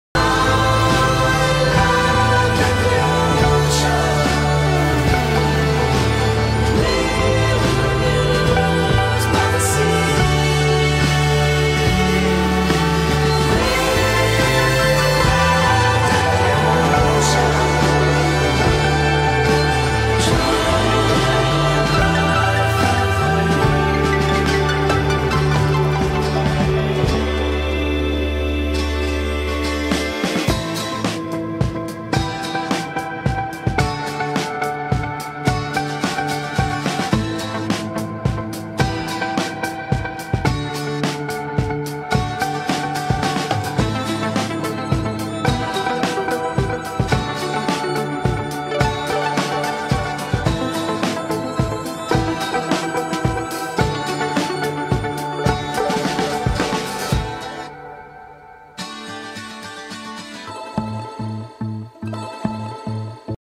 Swimming around Portimão Rock Formations
Sunny day on the 4th day of adventure through the Algarve, exploring the west side of Portimão ☀️☀️

Soon we’ll release the dates for Swim Camp Algarve 2025!